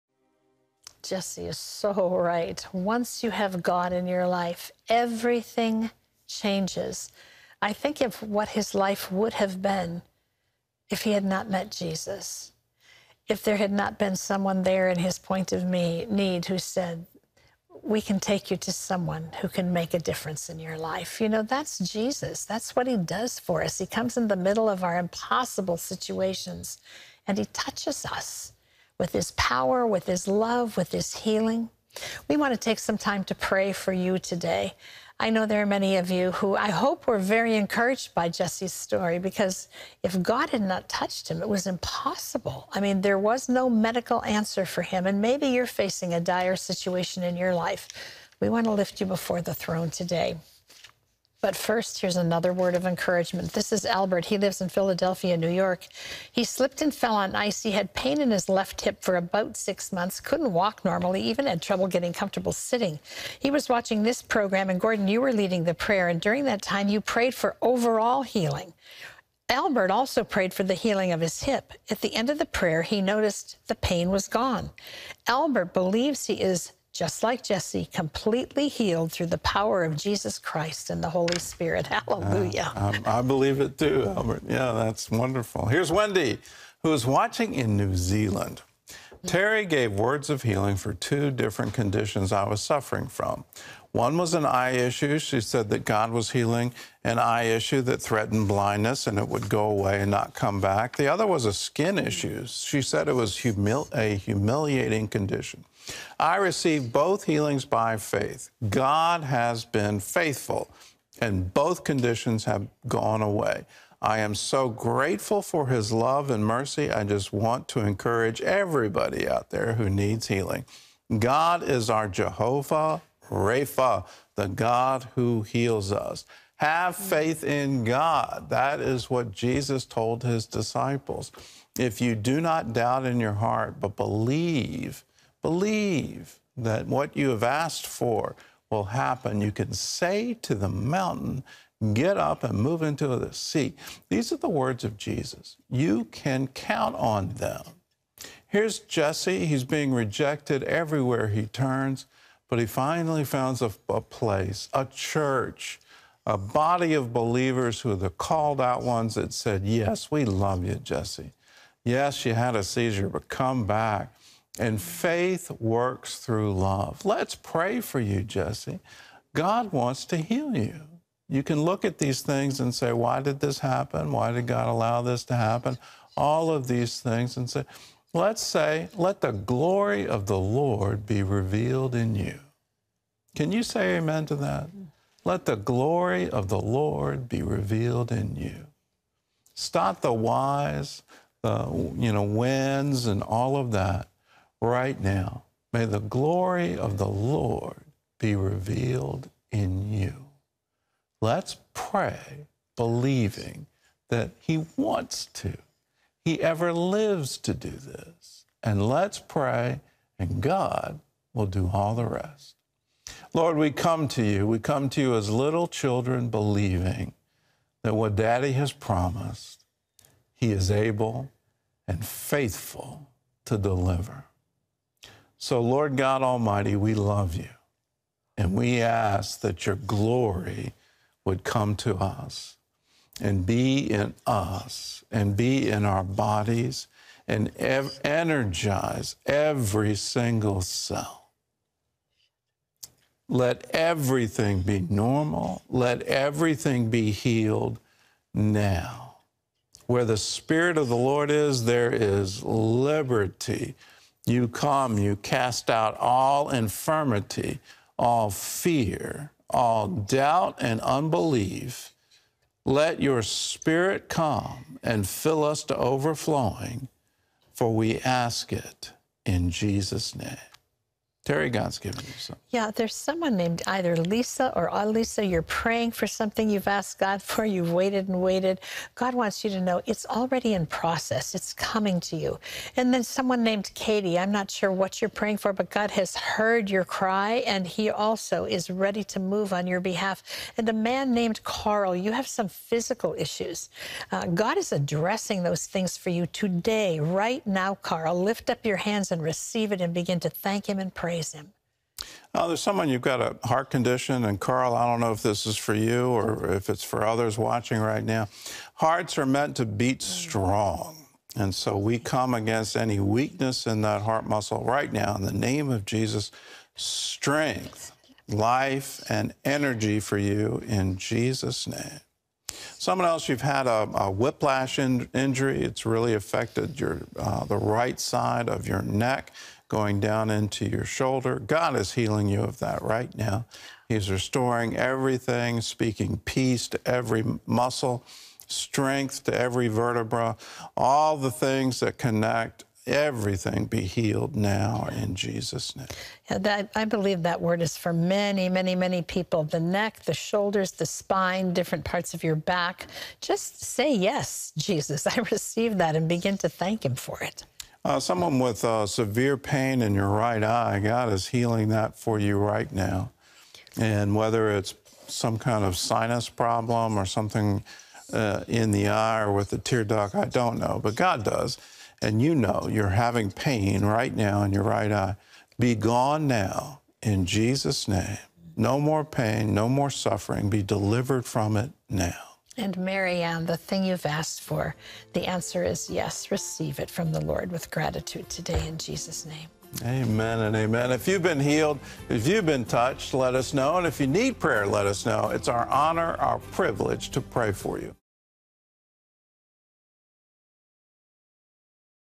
Praying For Your Needs: January 24, 2023
Gordon and Terry pray for you and deliver words of knowledge through the power of the Holy Spirit for your healing.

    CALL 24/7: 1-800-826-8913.